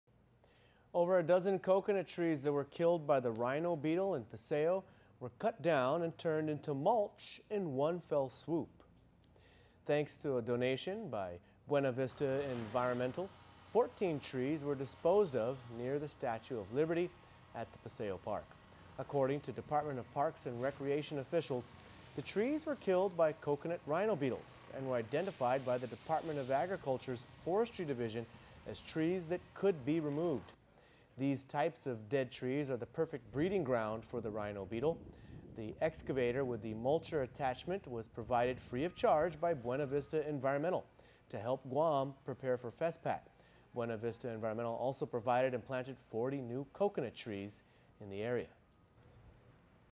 Coconut Trees at Paseo Cut Down & Turned into Mulch Because of Rhino Beetle
Buena Vista Environmental provided the service free of charge and also planted 40 coconut trees to replace the ones that had to be cut down.